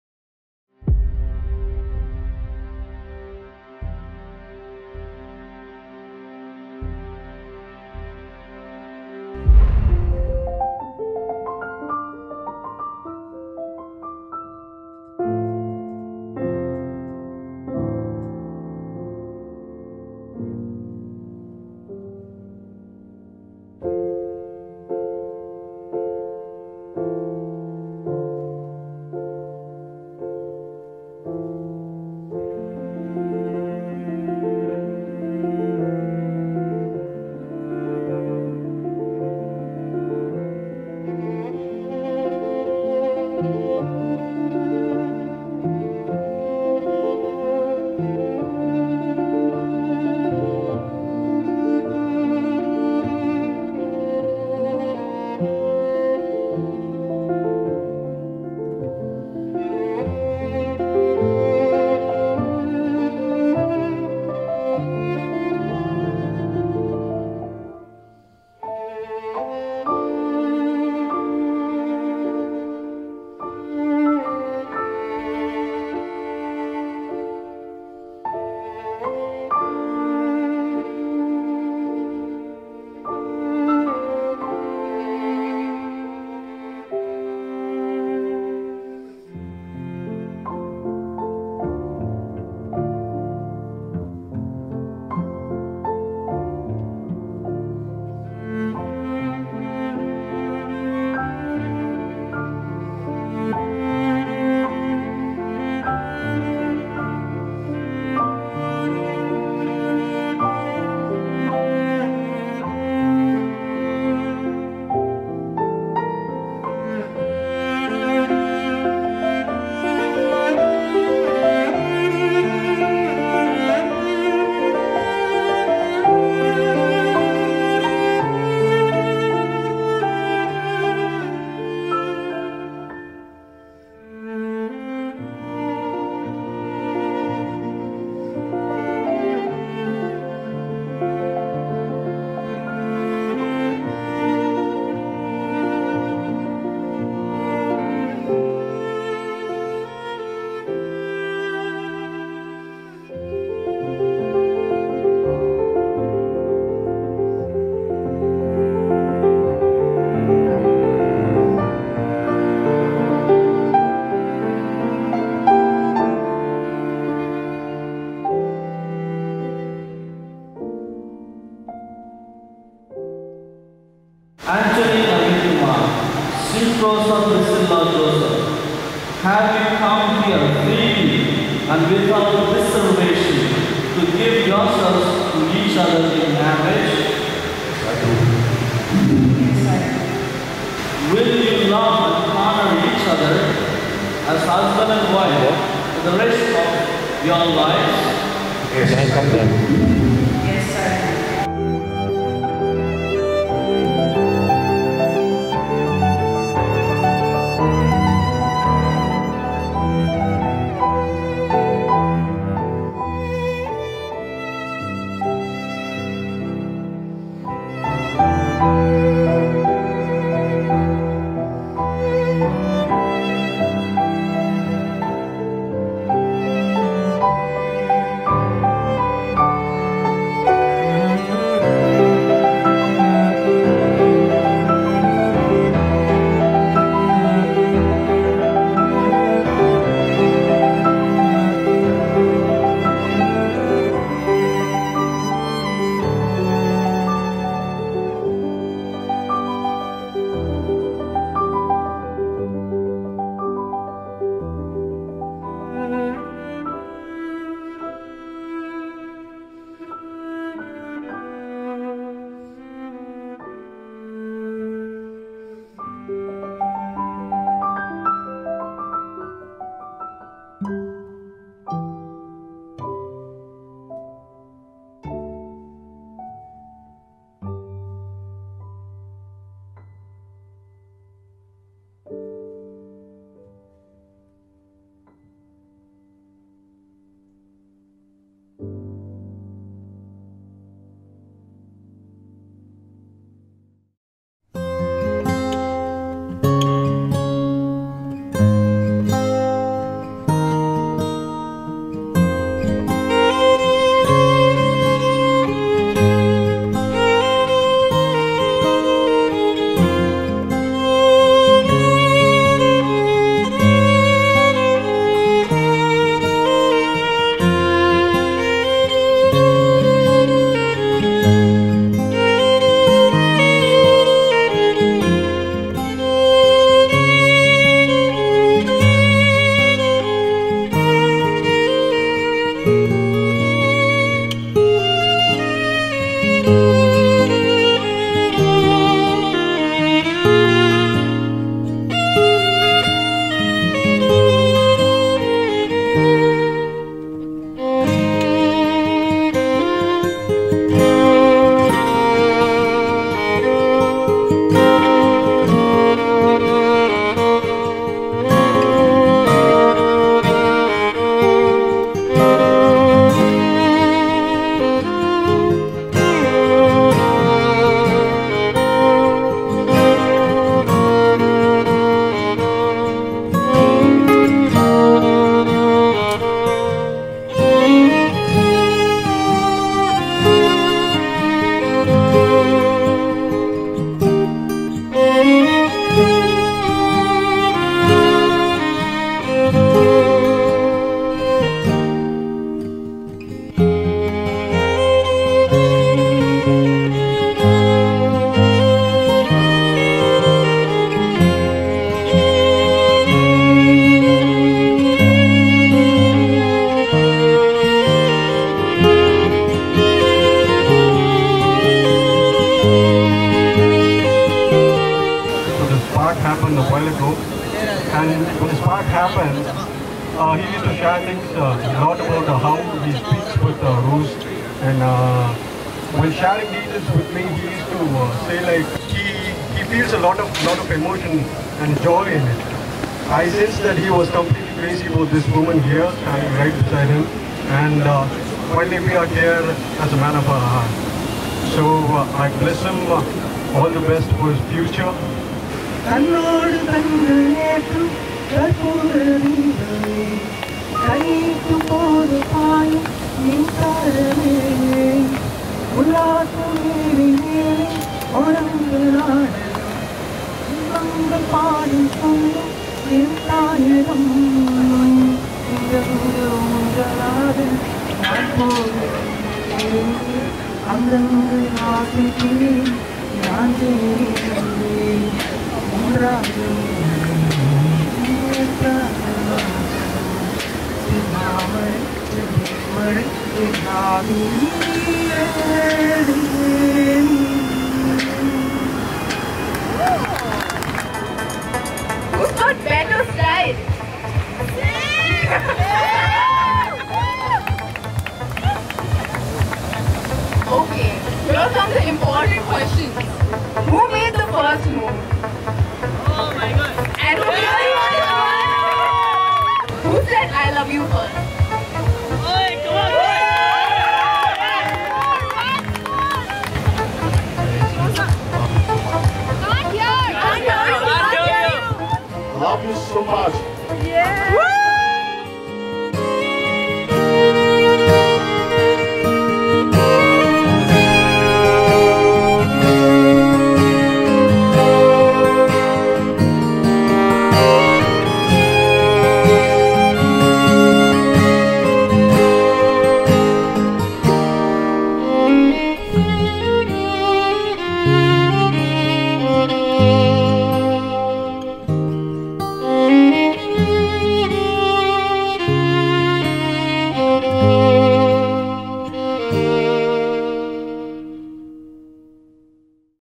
Symprosa & Antony
Nuptial Mass celebrated in National Shrine and Bascilica of St. Thomas on 7 Nov 2020